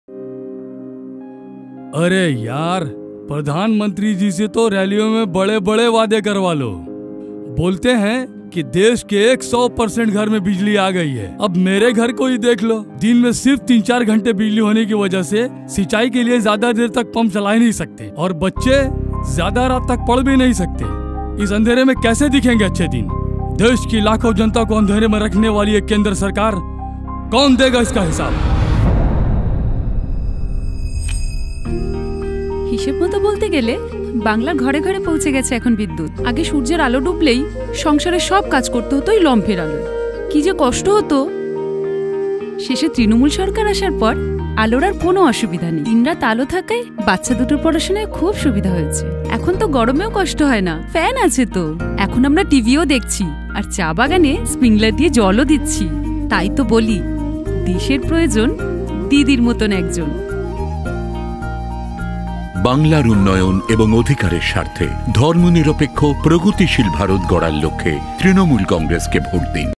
প্রধানমন্ত্রী হিসেব দাও
While some other parts of India are still in darkness, or get electricity for a few hours every day, Bengal has achieved 100% electrification.